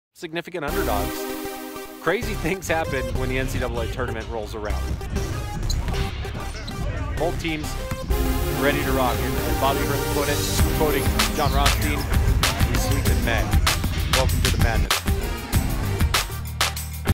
Two Arizona teams are left in the NCAA March Madness tournament
Two Arizona teams are left in the NCAA March Madness tournament, with No. 14 GCU playing No. 3 Gonzaga and No. 11 ASU playing No. 6 TCU. ABC15's Collin Harmon is in Denver with the preview. 🏀 🏆